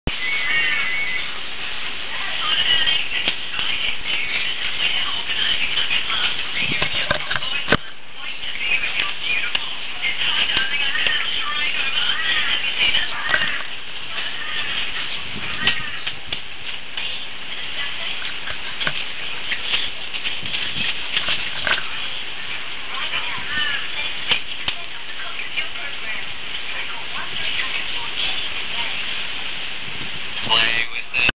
My two cats
just testing my new camera... disapointed in the quality but ..EH
3.1mp what should i expect.